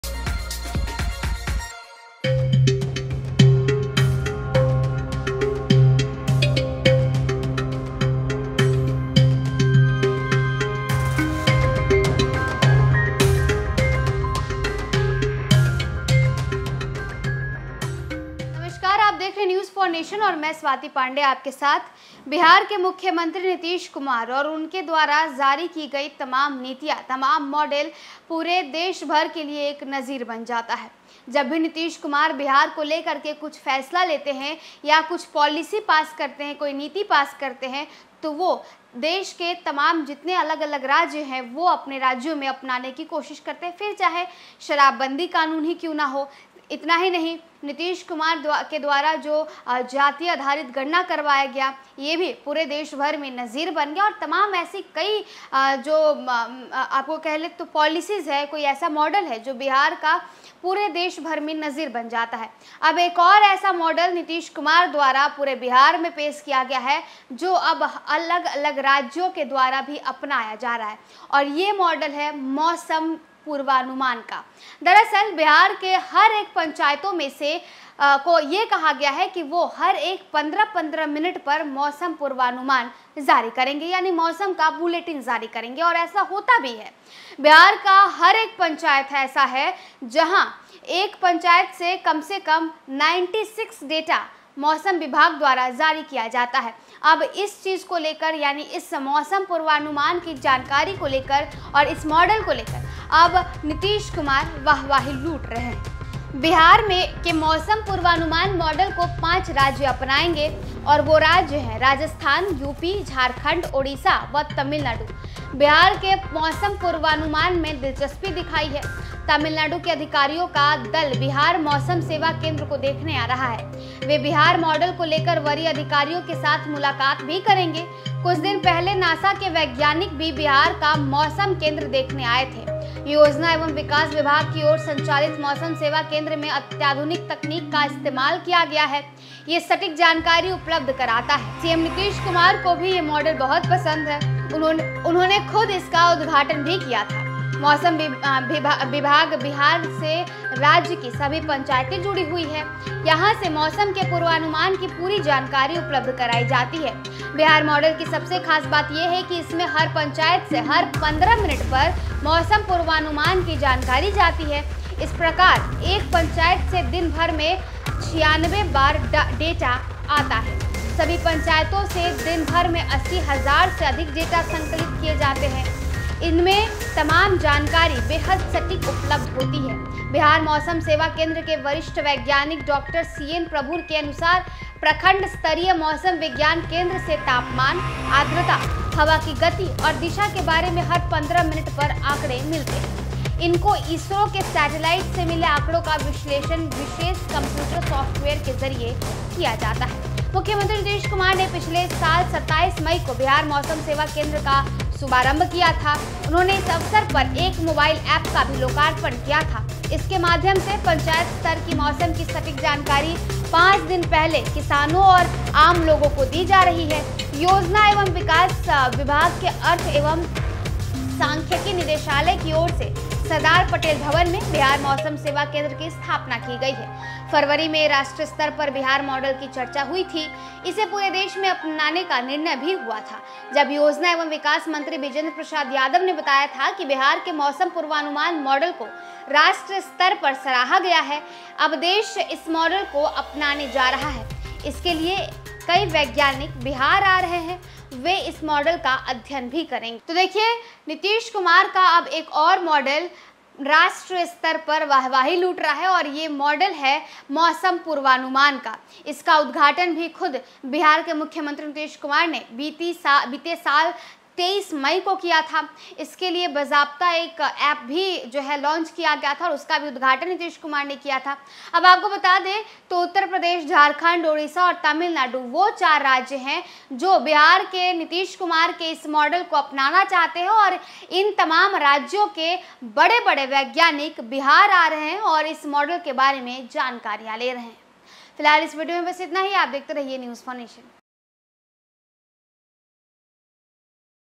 Nitish Kumar के इस काम का देश भर में बजने लगा डंका, सब अपना रहे कई राज्य आये सामने | Bihar News
Nitish Kumar के इस काम का देश भर में बजने लगा डंका, सब अपना रहे कई राज्य आये सामने |  Bihar News | News4Nation